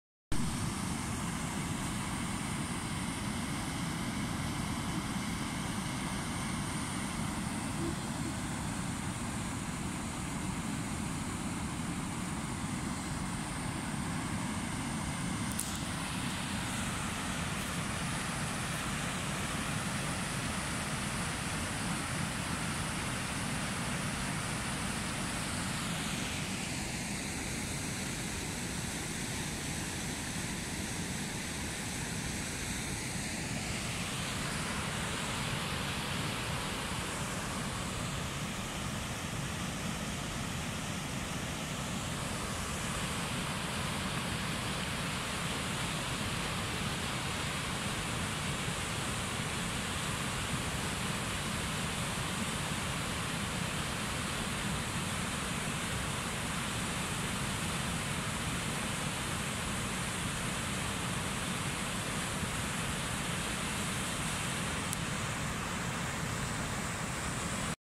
Rzeka Warta odcinek poniżej tamy na zbiorniku Jeziorsko w Łyszkowicach - Archiwum z dnia 11.10.2020
Archiwum 2020 - Brania słabe